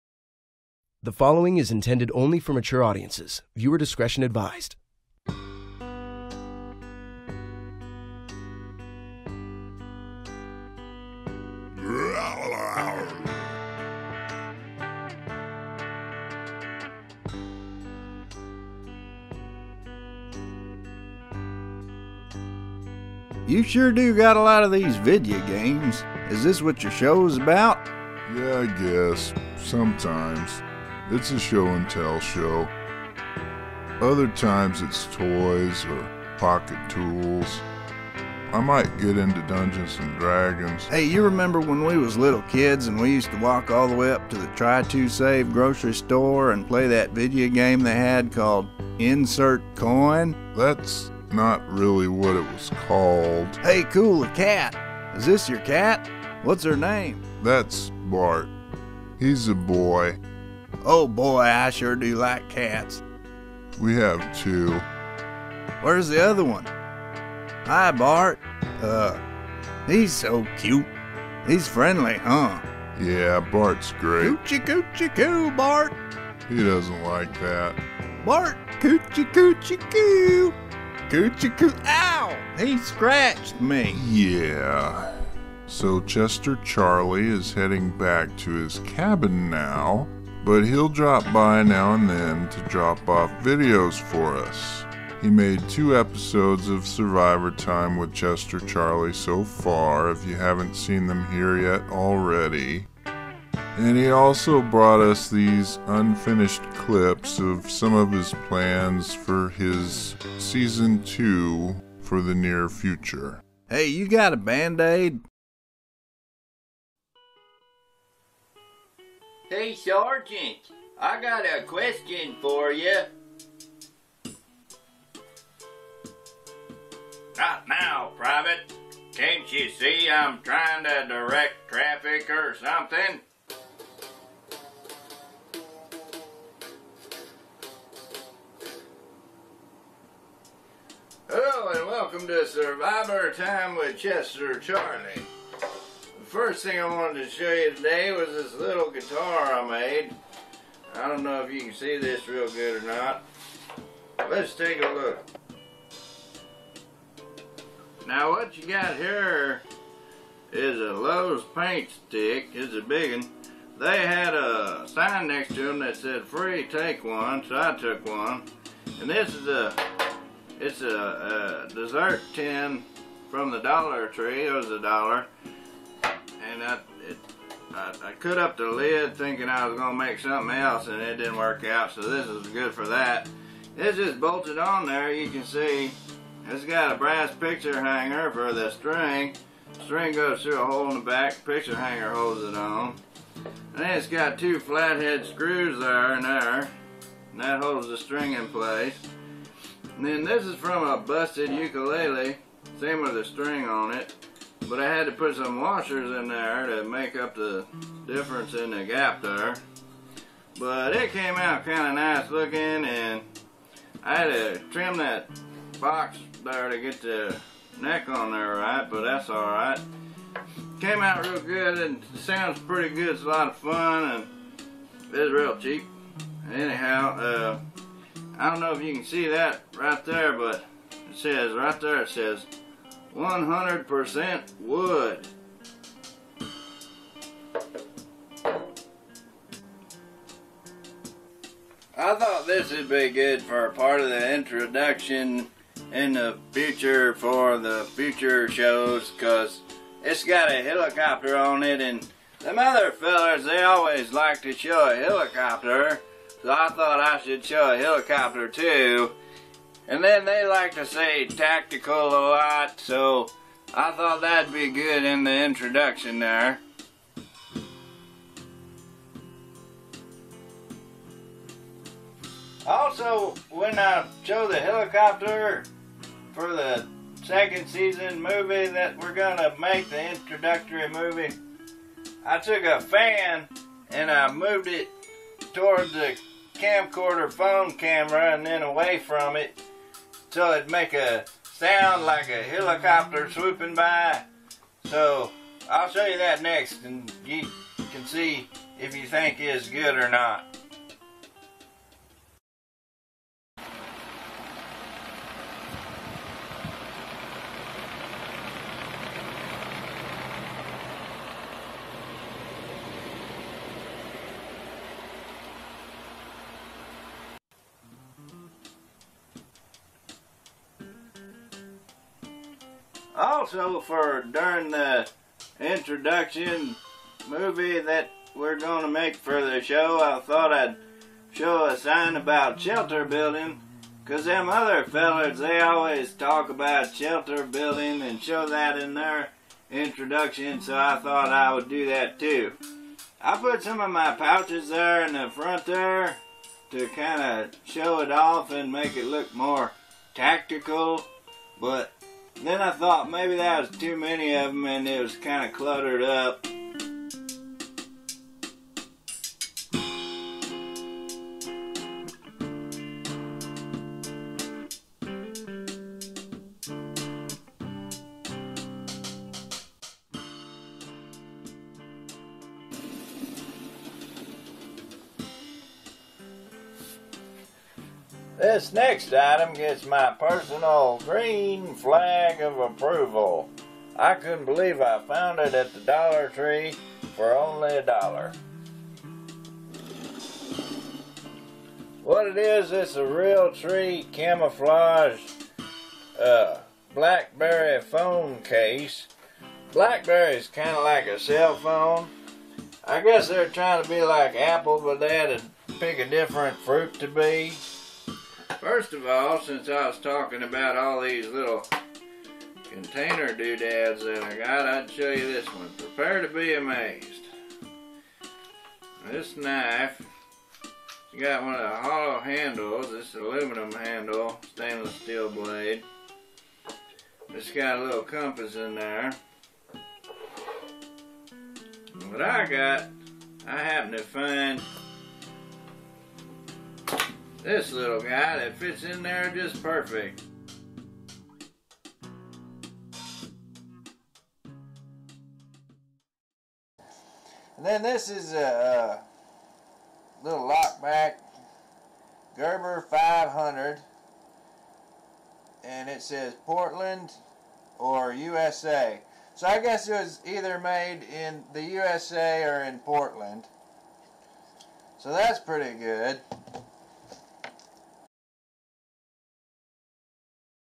Chester Charlie Clip Show
Before he returned to his cabin in the woods, Chester Charlie had a few more phone clips to share with us. Not completed episodes, but worth a look.
He considers it a preview of his future show.
I guess that means he'll be back sooner or later.